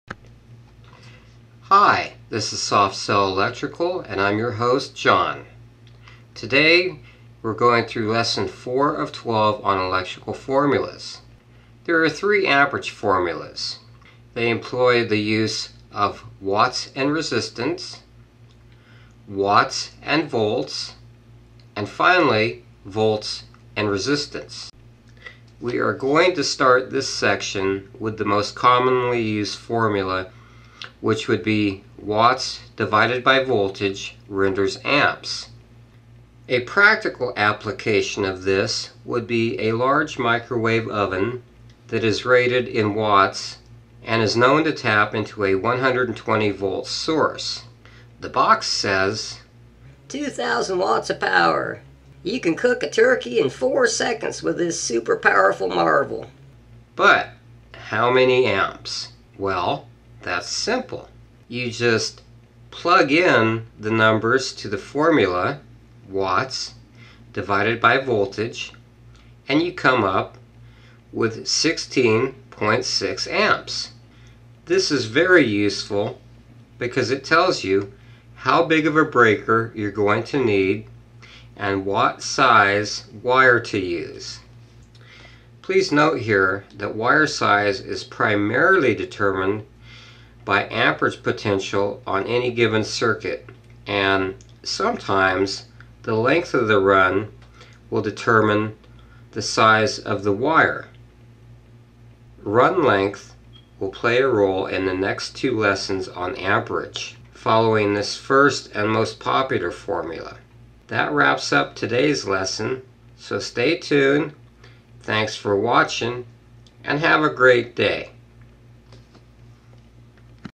How many amps do I need? #57
# 57 This is one of the more common formulas for determining how may amps an appliance will use. It is absolutely ESSENTIAL for determining wire and breaker size and will prevent many house fires when used to match the wire and breaker to the appliance to be used.